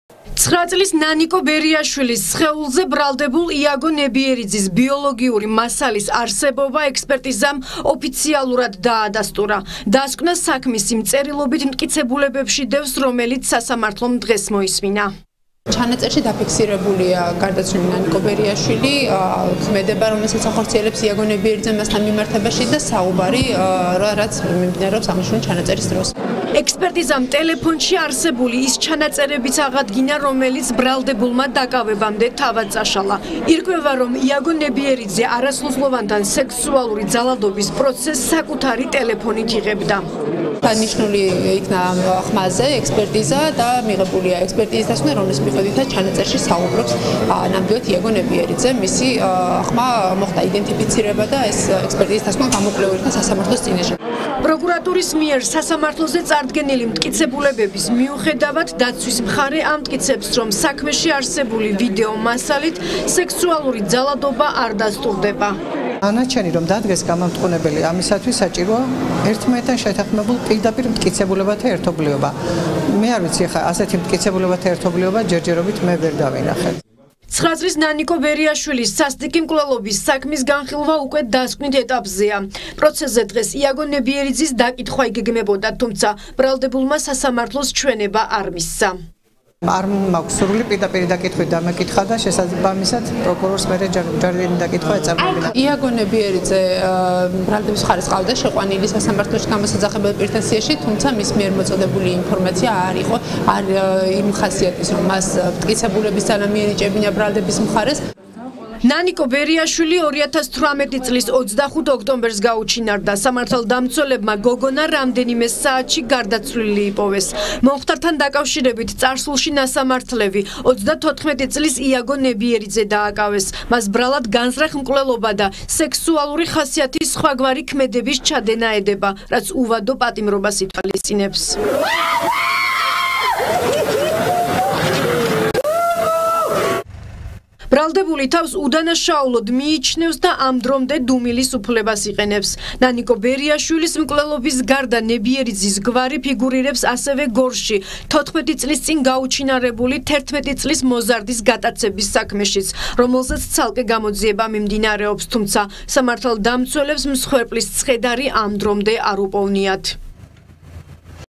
9 წლის ნანა ბერიაშვილის მკვლელობაში ბრალდებულ იაგო ნებიერიძეს შესაძლოა უვადო პატიმრობა მიესაჯოს
9 წლის ნანა ბერიაშვილის მკვლელობაში ბრალდებულ იაგო ნებიერიძეს შესაძლოა უვადო პატიმრობა მიესაჯოს